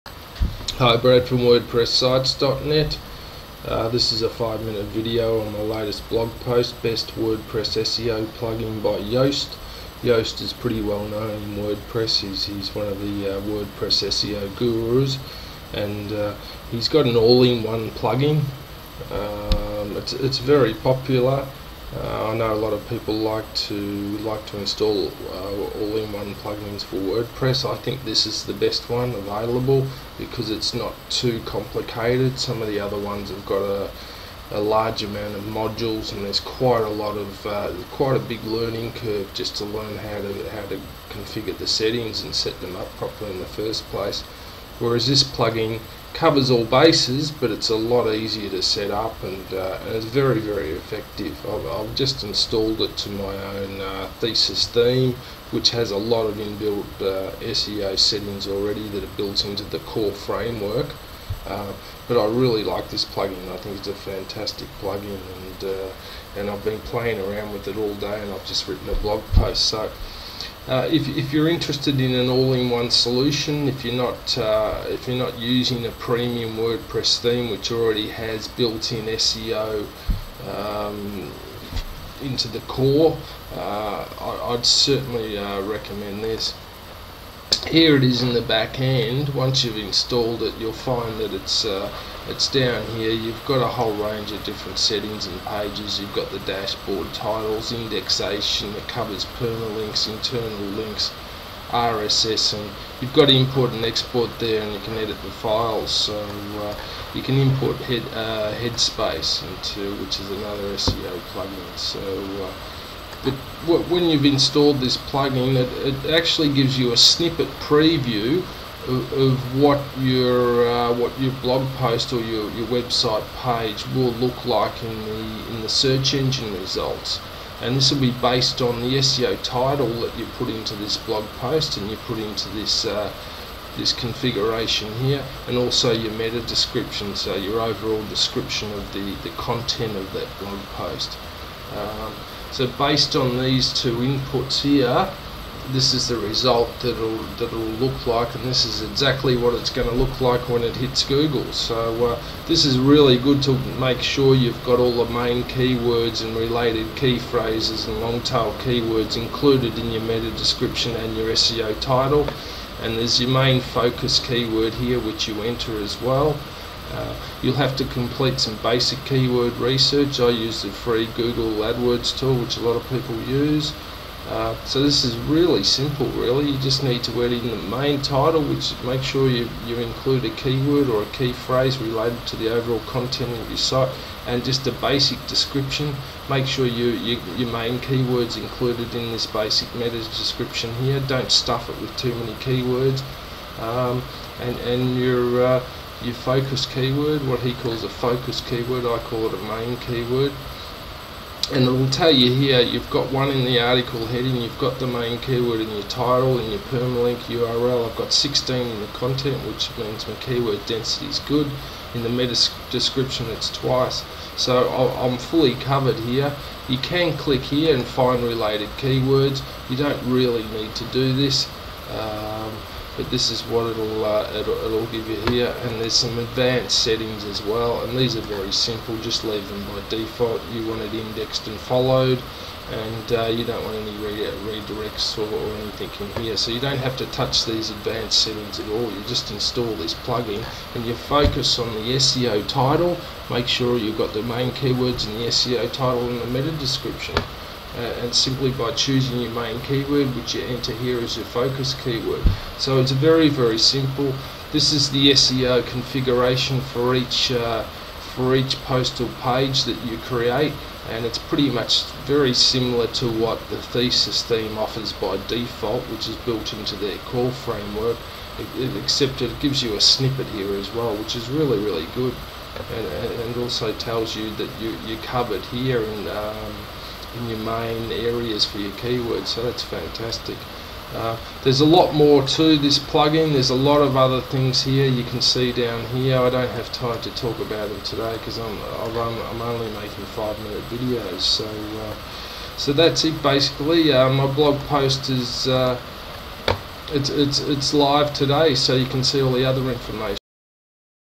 Best WordPress Seo Plugin by Yoast Offers an All in One Seo Solution For Your WordPress Blog
WordPress Tutorial on the Best WordPress Seo Plugin by Yoast Offers an All in One Seo Solution For Your WordPress Blog